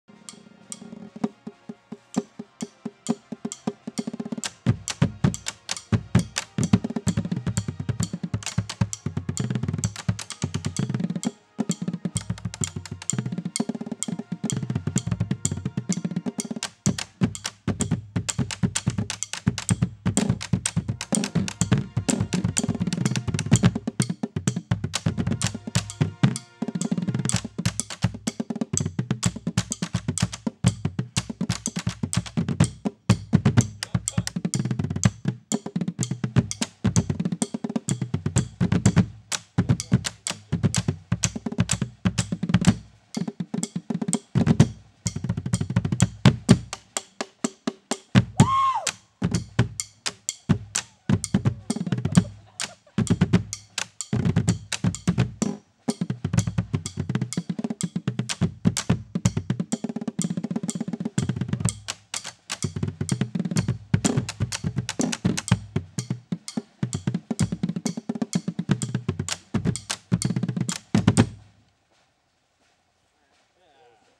2012 Phantom Bass Line - Bass Wok
Phantom Bassline plays the wok, watch in HD

Rockford,IL
7/15/12

Canon 5DmkII
24-105L